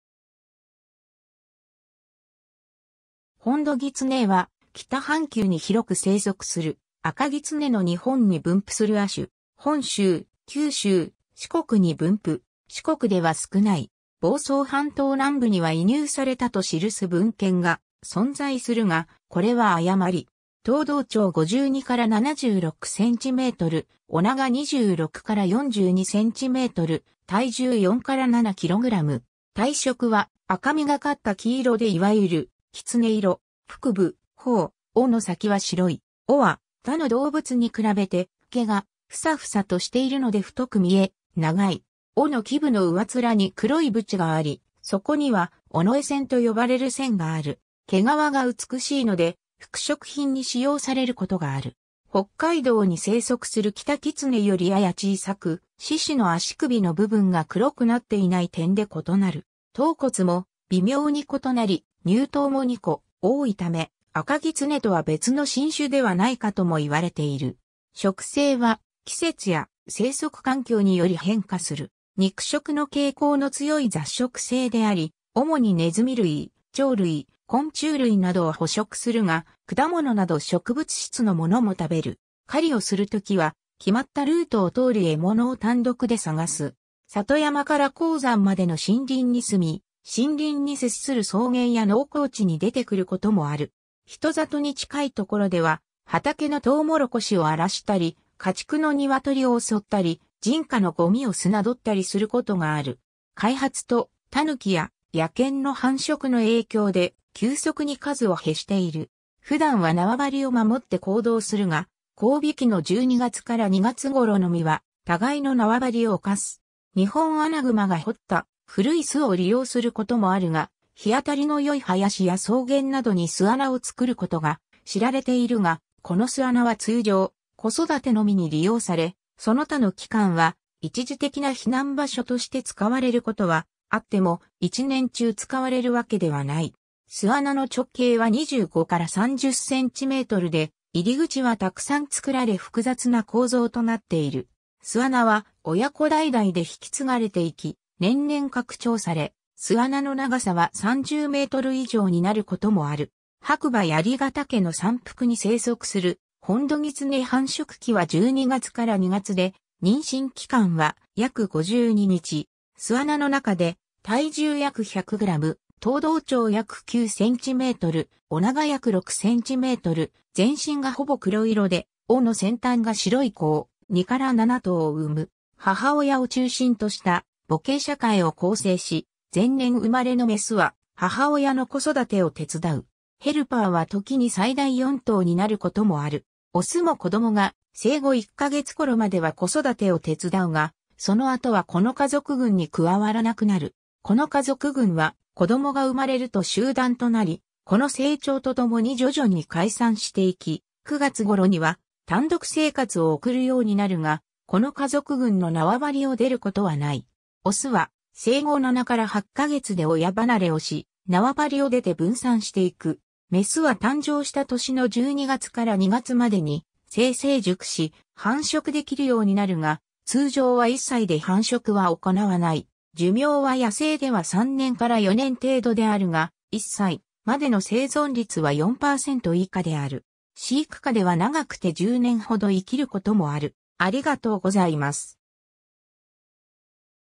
ホンドギツネ
ホンドギツネ（本土狐、Vulpes vulpes japonica）は、北半球に広く生息するアカギツネの日本に分布する亜種。
 本州、九州、四国に分布。
四国では少ない。
房総半島南部には移入されたと記す文献が存在するが、これは誤り。
 頭胴長52-76 cm、尾長26-42 cm、体重4-7 kg。
体色は赤みがかった黄色でいわゆる「きつね色」。
腹部、頬、尾の先は白い。
尾は他の動物に比べて毛がふさふさとしているので太く見え、長い。
尾の基部の上面に黒い斑があり、そこには尾上腺（尾腺）と呼ばれる腺がある。
毛皮が美しいので、服飾品に使用されることがある。
北海道に生息するキタキツネよりやや小さく、四肢の足首の部分が黒くなっていない点で異なる。
頭骨も微妙に異なり、乳頭も2個多いため、アカギツネとは別の新種ではないかとも言われている。
 食性は季節や生息環境により変化する。
肉食の傾向の強い雑食性であり、主にネズミ類、鳥類、昆虫類などを捕食するが果物など植物質のものも食べる。
狩りをする時は、決まったルートを通り獲物を単独 で探す。
里山から高山までの森林に住み、森林に接する草原や農耕地に出てくることもある。
人里に近い所では、畑のトウモロコシを荒らしたり、家畜のニワトリを襲ったり、人家のゴミを漁ったりすることがある。
開発とタヌキや野犬の繁殖の影響で、急速に数を減している。
 普段は縄張りを守って行動するが、交尾期の12月から2月ごろのみは互いの縄張りを侵す。
ニホンアナグマが掘った古い巣を利用することもあるが、日当たりの良い林や草原などに巣穴を作ることが知られているが、この巣穴は通常、子育てのみに利用され、その他の期間は、一時的な避難場所として使われることはあっても、一年中使われるわけではない。
巣穴の直径は25-30cmで、入り口はたくさん作られ複雑な構造となっている。
巣穴は親子代々で引き継がれていき、年々拡張され、巣穴の長さは30m以上になることもある。
 白馬鑓ヶ岳の山腹に生息するホンドギツネ 繁殖期は12月から2月で、妊娠期間は約52日。
巣穴の中で体重約100 g、頭胴長約9 cm、尾長約6 cm、全身がほぼ黒色で尾の先端が白い子を、2-7頭産む。
母親を中心とした母系社会を構成し、前年生まれのメス（ヘルパー）は母親の子育てを手伝う。
ヘルパーは時に最大4頭になることもある。
オスも子供が生後1ヶ月頃までは子育てを手伝うが、その後はこの家族群に加わらなくなる。
この家族群は、子供が生まれると集団となり、子の成長とともに徐々に解散していき、9月頃には単独生活を送るようになるが、この家族群のなわばりを出ることはない。
 オスは生後7-8ヶ月で親離れをし、なわばりを出て分散していく。
メスは誕生した年の12月から2月までに性成熟し、繁殖できるようになるが、通常は1歳で繁殖は行わない。
寿命は野生では3年から4年程度であるが、1歳までの生存率は4%以下である。
飼育下では長くて10年ほど生きることもある。